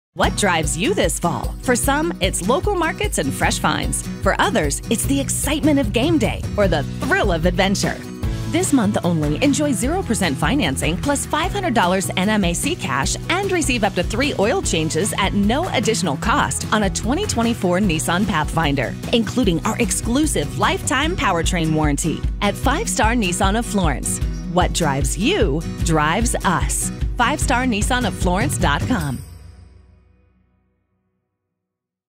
What Drives You, Drives Us
What drives you this fall?

For some, it's local markets and fresh finds. For others, it's the excitement of game day, or the thrill of adventure.

Right now, get 0% financing plus $500 NMAC cash and receive up to 3 oil changes at no additional cost on a new 2024 Nissan Pathfinder, including our exclusive Lifetime Powertrain Warranty.

At Five Star Nissan of Florence, what drives you, drives us.